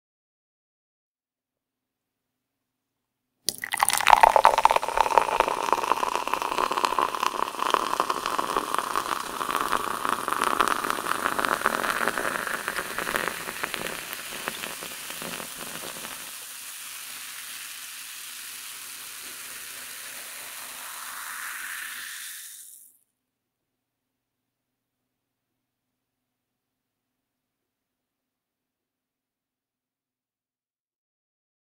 SFX: FILL A GLAS WITH ENERGY DRINK (incl. BUBBLE SOUND) - EIN GLAS MIT ENERGYDRINK FÜLLEN
DE:  EIN GLAS MIT ENERGYDRINK FÜLLEN (inkl. PERLENBLÄSCHEN SOUND)
EN: FILL A GLAS WITH ENERGY DRINK (incl. BUBBLE SOUND)